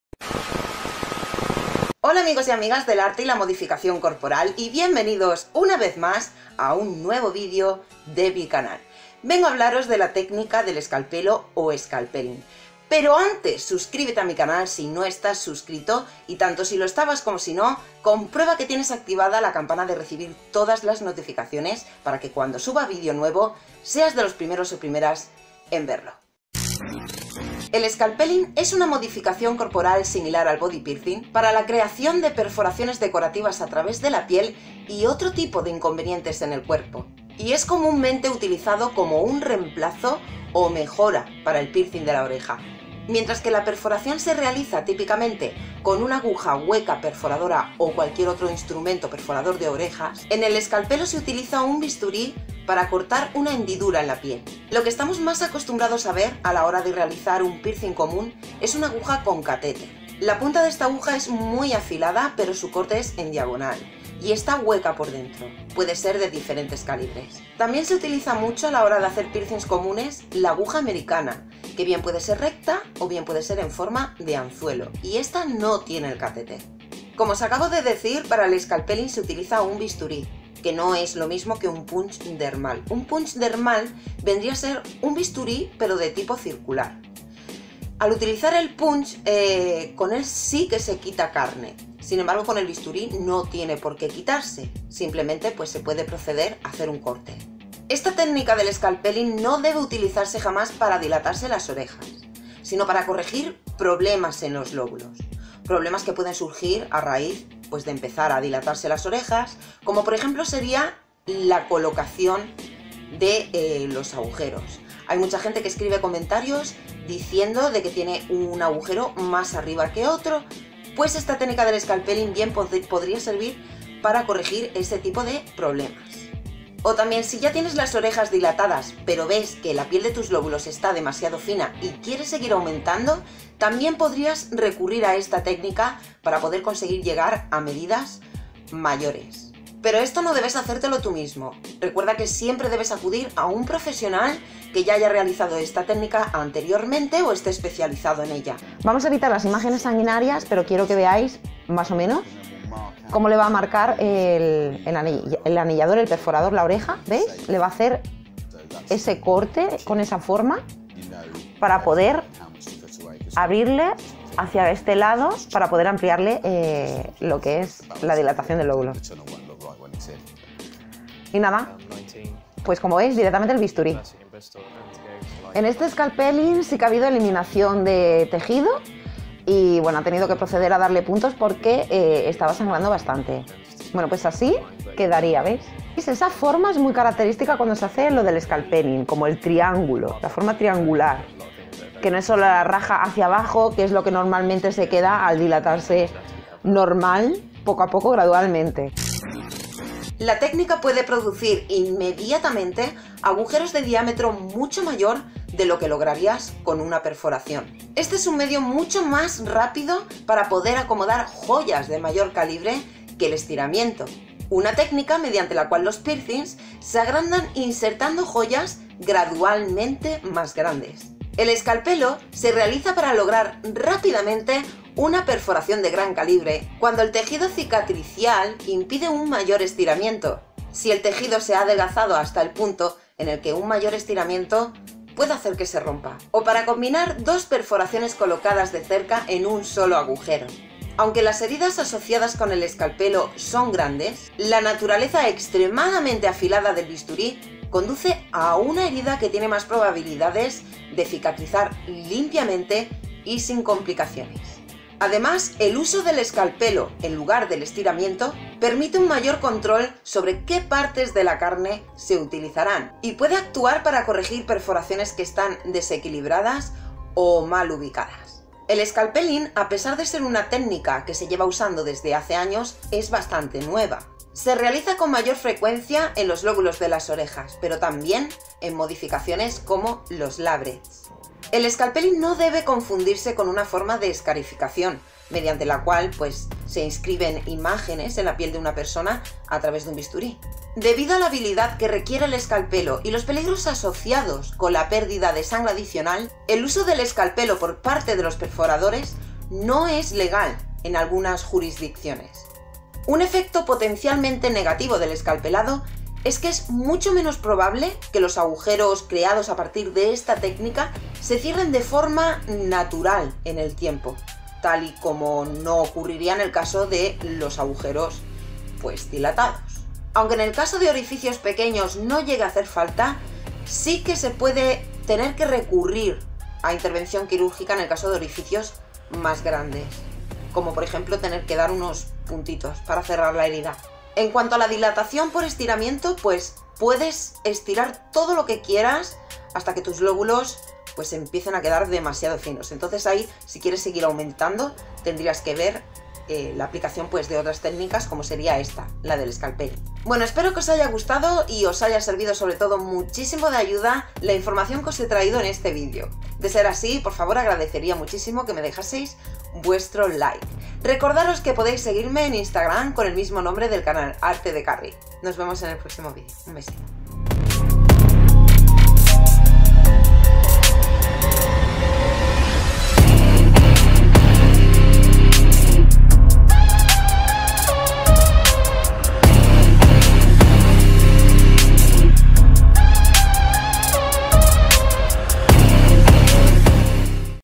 🔥 SCALPELLING EN DILATACIONES DE OREJA ¿Qué es? ¿Para qué sirve?
⚡➡️¿Tienes las dilataciones desviadas? ¿Lóbulos demasiado finos? ¿Expansiones deformadas? Te explico en qué consiste la técnica del escalpelado por sí no sabes de ella o quieres más información.